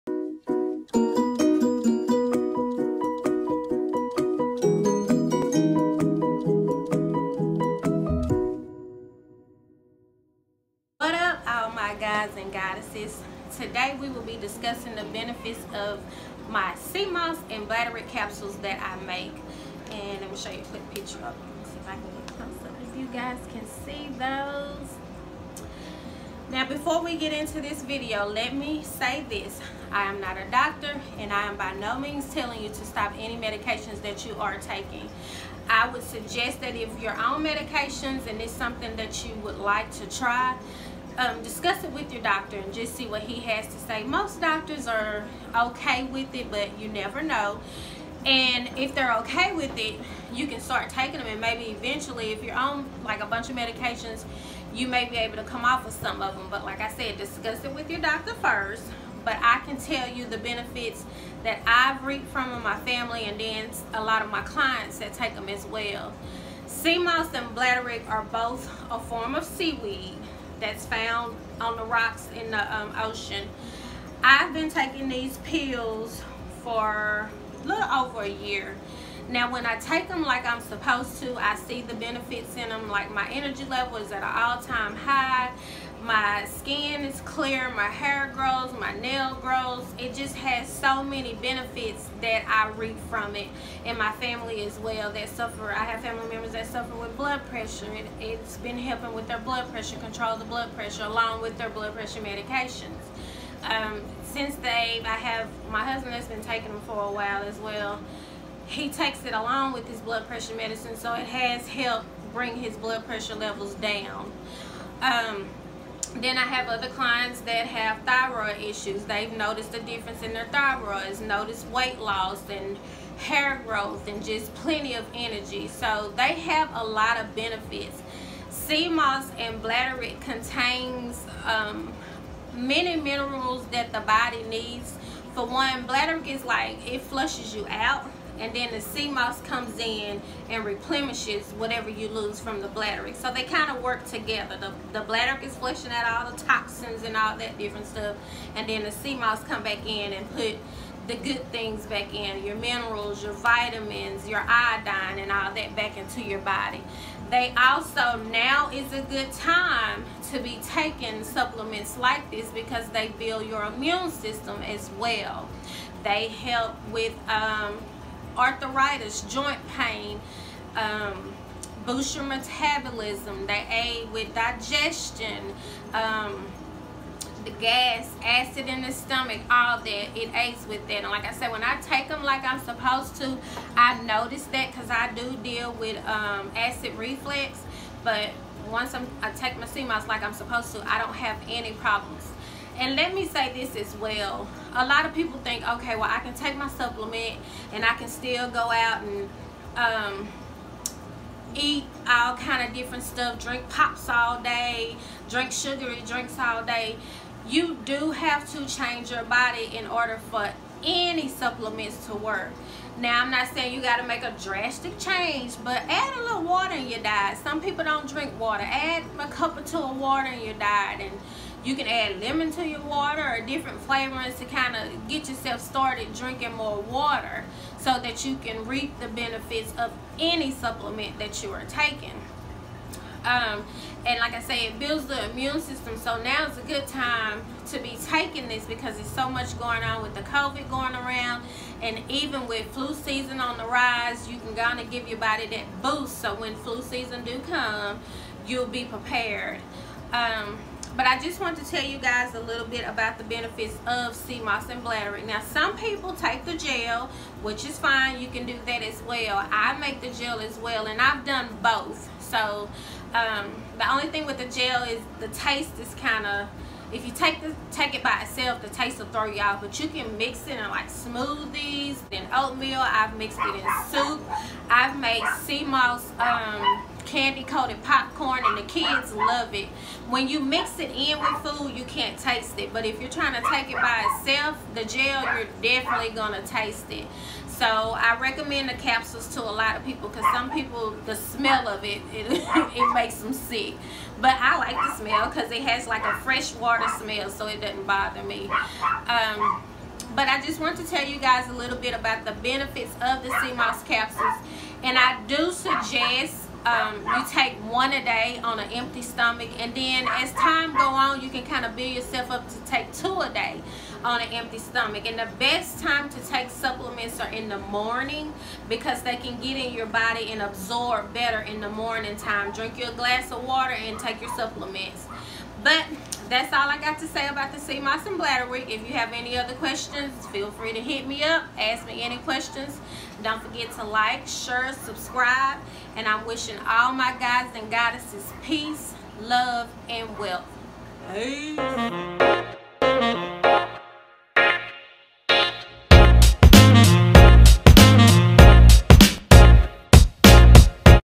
Benefits of seamoss and bladderwrack capsules
Seamoss and Bladderwrack are a form of seaweed that are loaded with minerals and vitamins. I will discuss with you the benefits of taking these capsules and how to take them.

Etsy shop